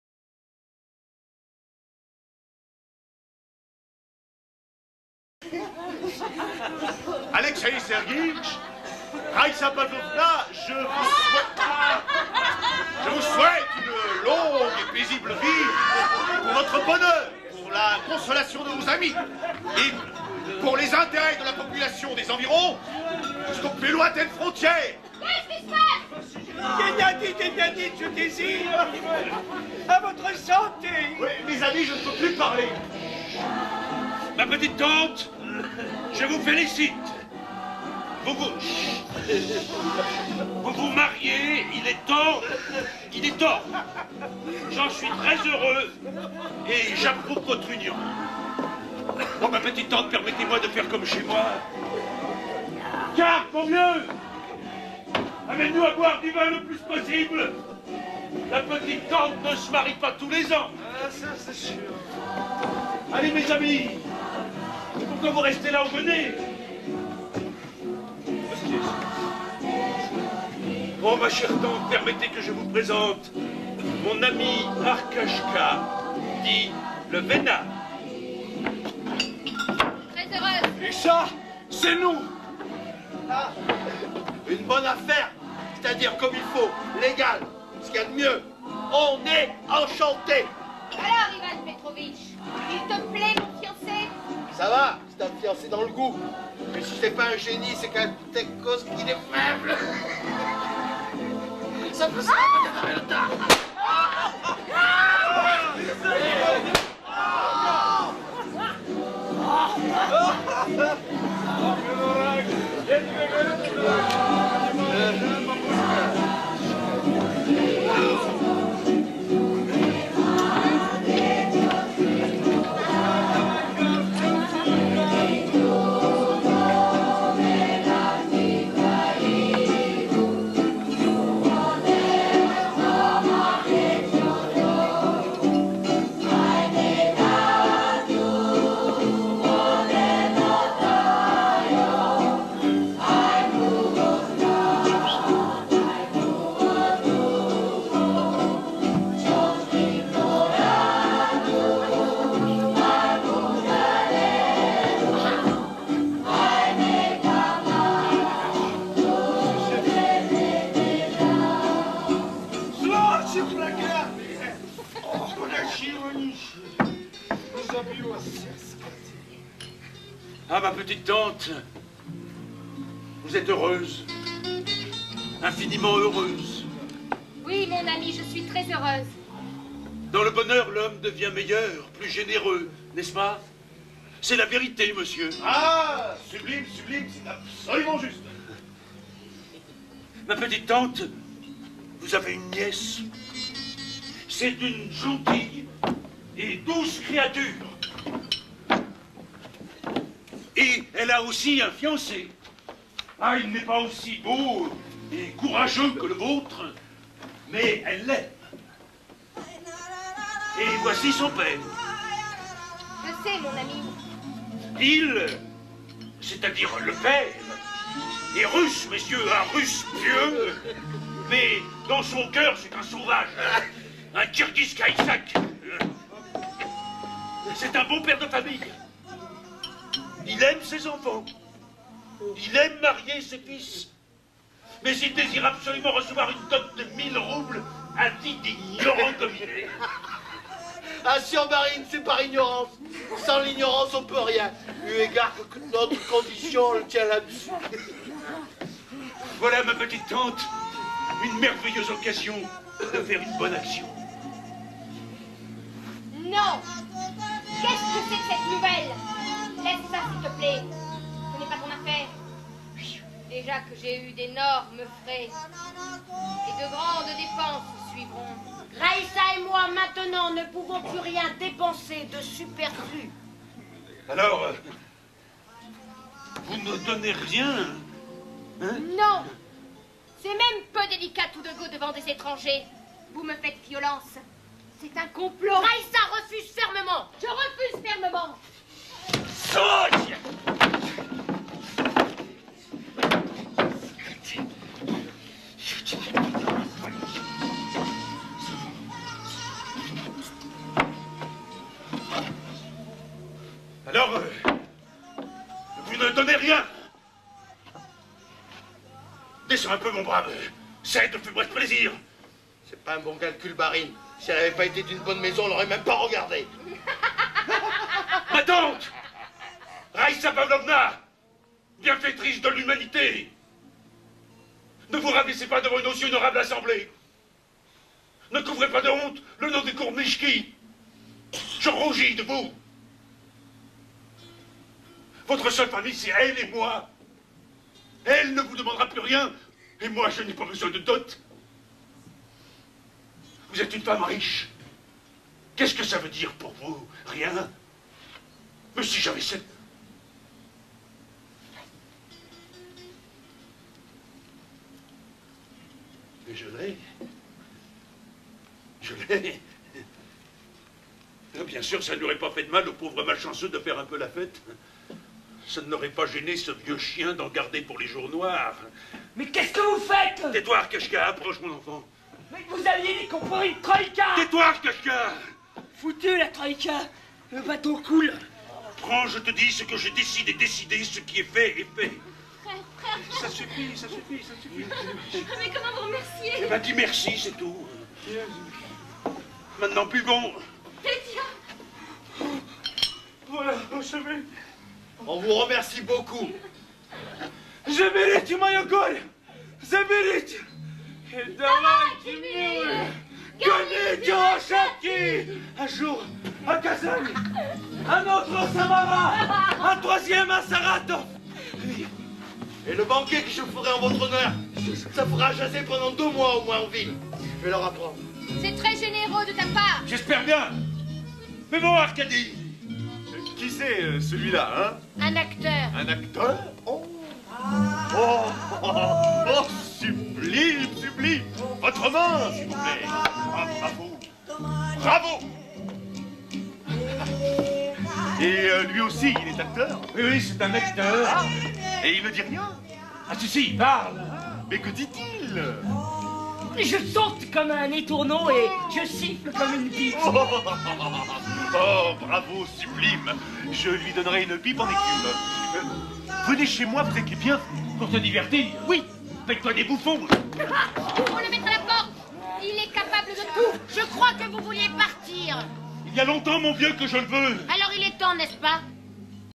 La Forêt7-Ostrovski
La Forêt d'Ostrovski mise en scène par Cyril Grosse (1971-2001) et créée en 1999 à la Maison des Comoni au Revest avec L'Insolite Traversée: 10 comédiens sur le plateau. Ce spectacle a tourné 2 ans et a valu à Cyril Grosse d'être conventionné par l'État. Dans cette scène Katia Ponomareva, la soeur de Cyril G. joue madame Gourmischkaya et se marie avec Alexeiev (Magali Bonat). Avec Laurent Chouteau, Valérie Marinèse, Magali Bonat, Pierre Palmi, Bernard Meulien, Jeanne Mathis, Victor Ponomarev, Katia Ponomareva-Grosse, Laurent Vercelletto et Cyril Grosse.